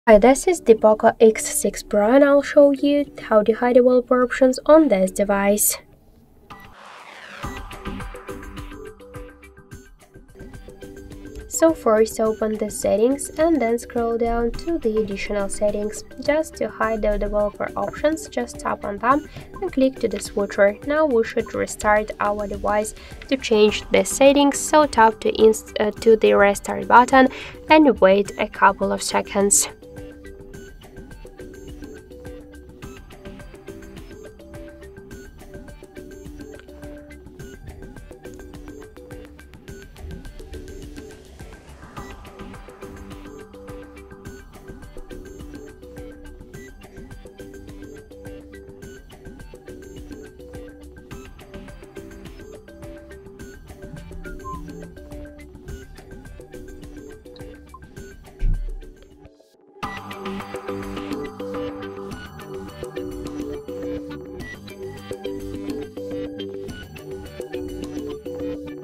How to Hide Developer Options In Poco X6 Pro
Developer options provide advanced settings and debugging tools for users who are familiar with software development. However, for most users, these options may not be necessary and can clutter the settings menu.

What are developer options, and why are they included in smartphones like the Poco X6 Pro?
Can you explain the potential risks or drawbacks of leaving developer options visible in the settings menu?
Where can users find developer options on their Poco X6 Pro devices, and why might they want to hide them?
What steps are involved in hiding developer options on the Poco X6 Pro, and are there any prerequisites or considerations users should be aware of?